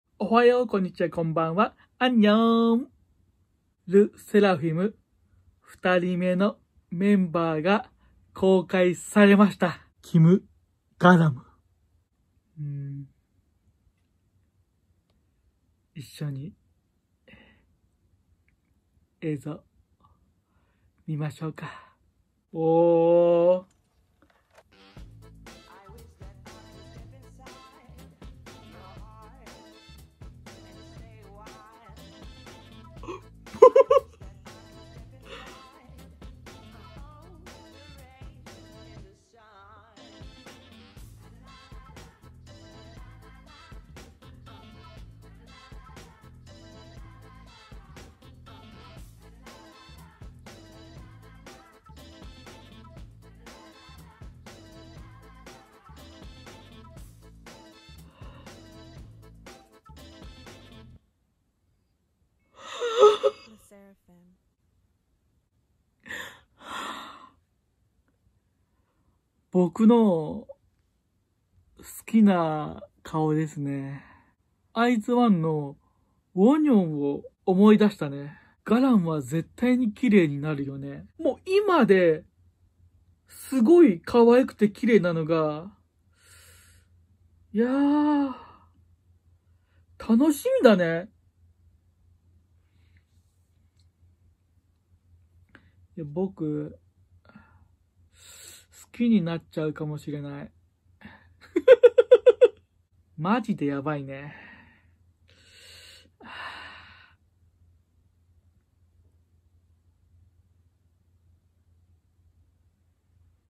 【LE SSERAFIM/르세라핌】Reaction: "KIM GARAM" PV / 리액션: 김가람... 난 사랑에 빠졌어요!【한국어 자막】
진짜 대박❤️マジでヤバい❤️
사랑에 빠지다😍恋に落ちる😍
함께 응원합시다🤝
一緒に応援しましょう🤝